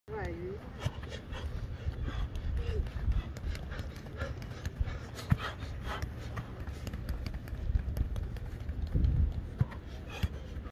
Yassin Ayoub Bij Soccermoves!!
Yassin Ayoub aan het trainen!
Ayoub is op vakantie in Nederland en zelfs tijdens zijn winterstop traint hij door dit laat ook zijn gedrevenheid zien.

Wij wensen Ayoub veel succes bij Panathinaikos!!